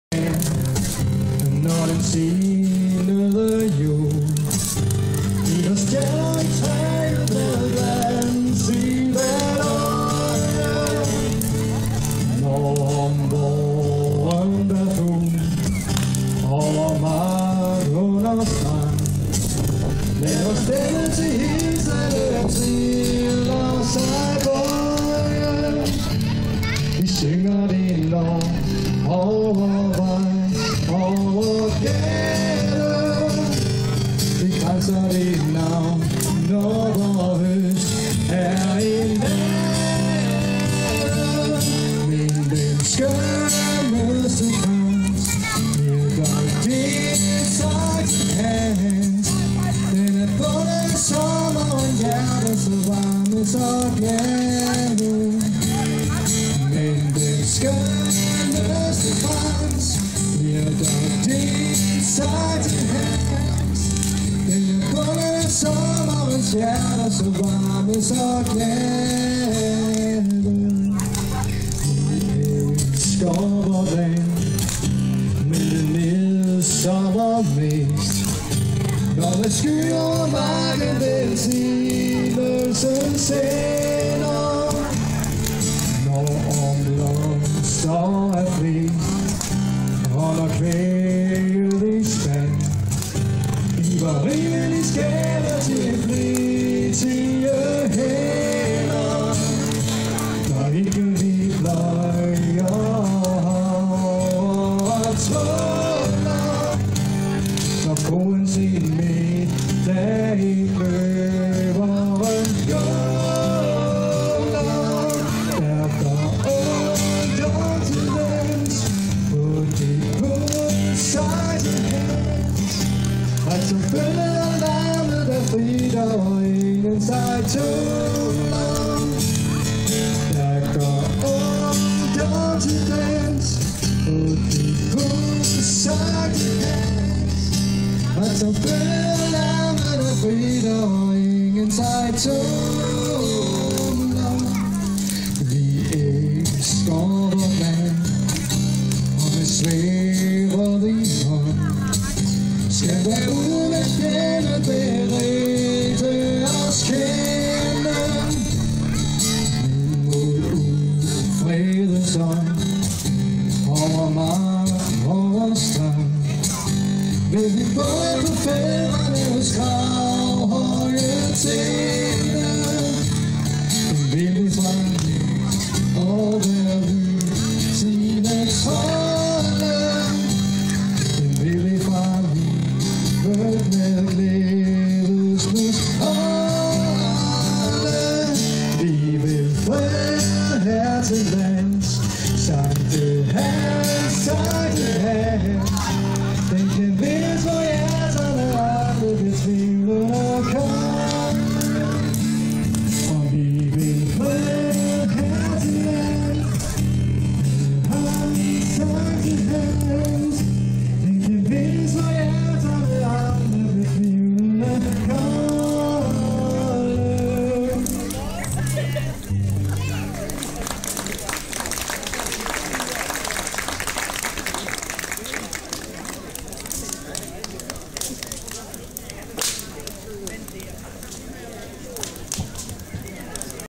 Sankt Hans Aften ved Harreslev Danske Kirke 2012
En dejlig og hyggelig Sankt Hans Aften ved Harreslev Danske Kirke med live musik, masser af snak og godt humør.